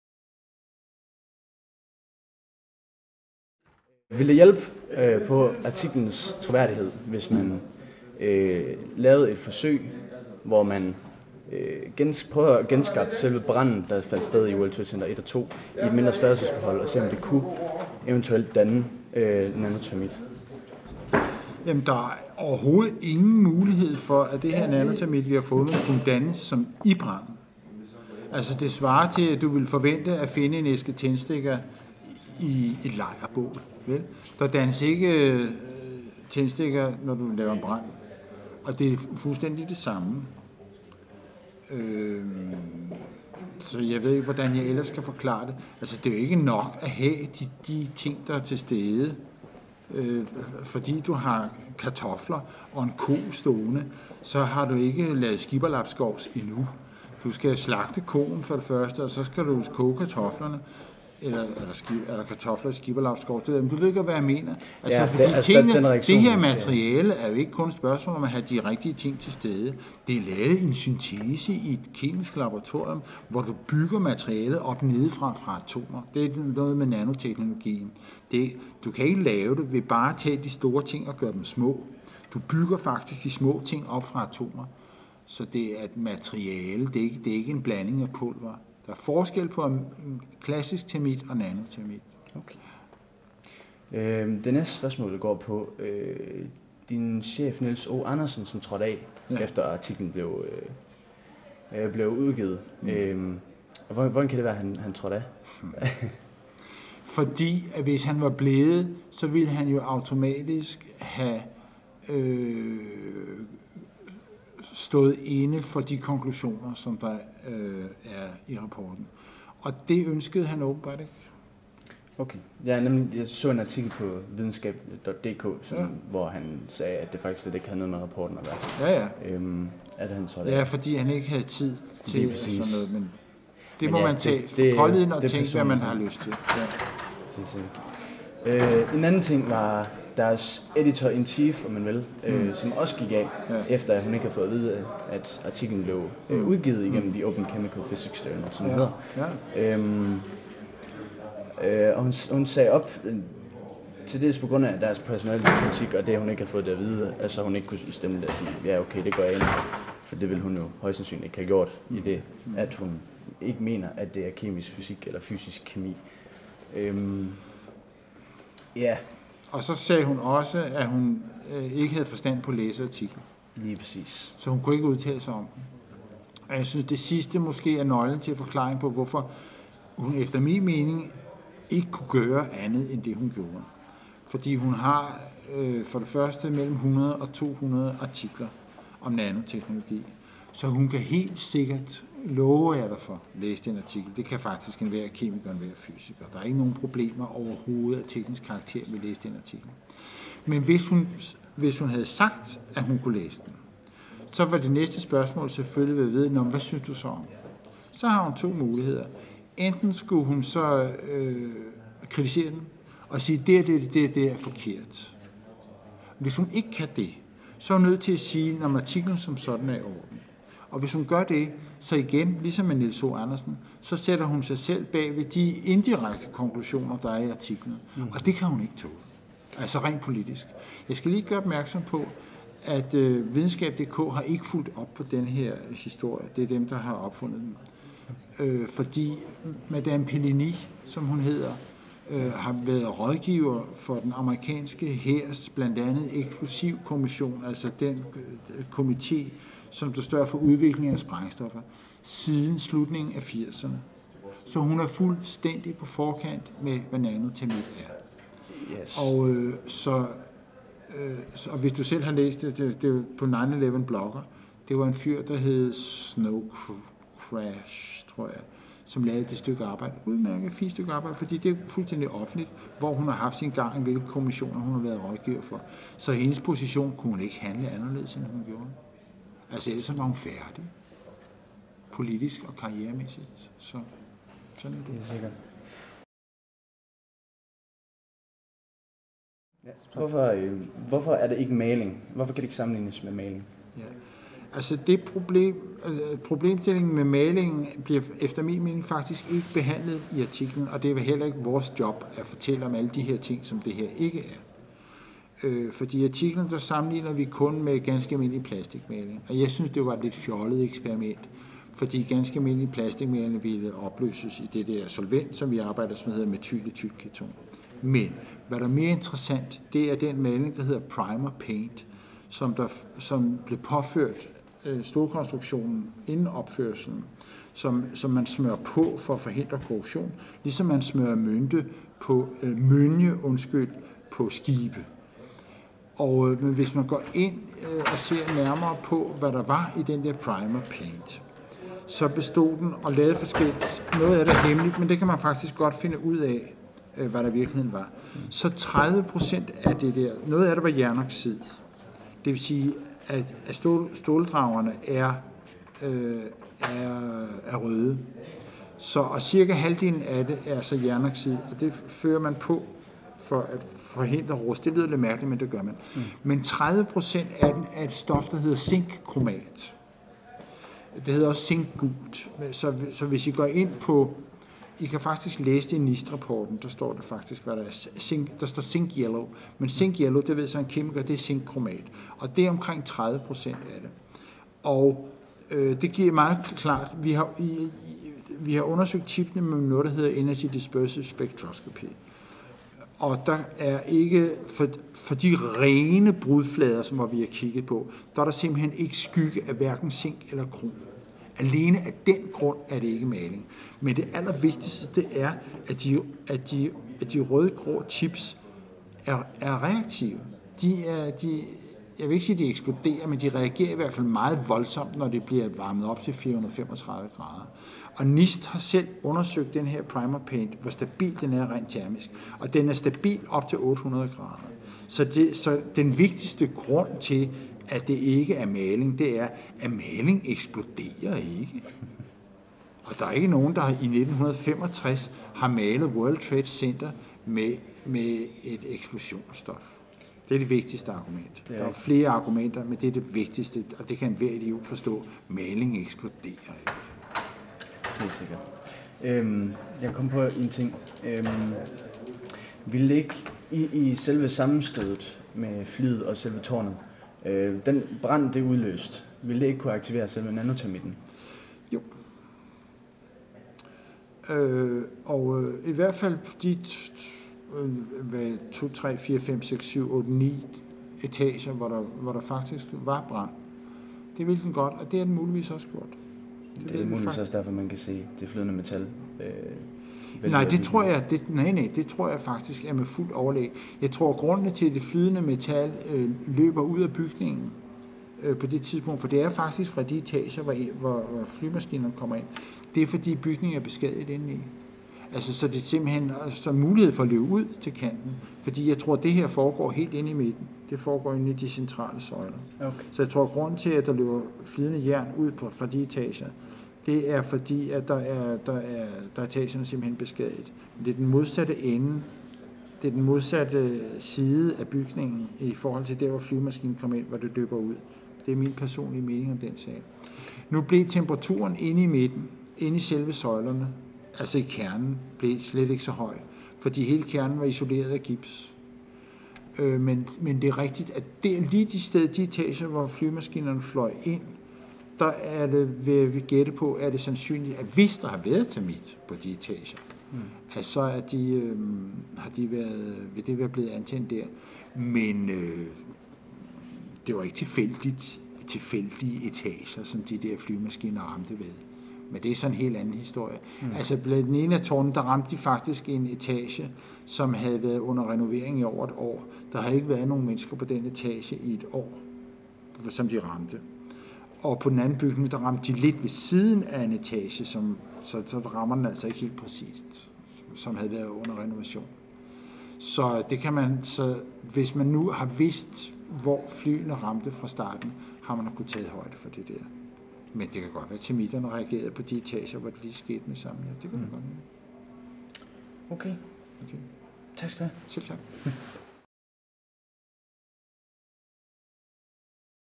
Niels Harrit Interview om sin rapport
Efter Niels Harrits foredrag på Studenterhuset i Aalborg d. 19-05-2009, fik vi en kort snak med Niels Harrit om hans artikel, "Active Thermitic Material Discovered in Dust from the 9/11 World Trade Center Catastrophe", i tidsskriftet "The Open Chemical Physics Journal".

Artiklen har skabt meget omtale, da det er det første videnskabelige bevis for sprængstoffer i forbindelse med 9/11 katastrofen. Vi samlede de mest interessante spørgsmål vi kunne finde på de videnskabelige hjemmesider der har dækket Niels Harrits artikel, og spurgte ham om hvad han havde at sige til det.

Interviewet varer cirka 10 min. hvor den starter med troværdigheden bag artiklen og fundet af nano-termit.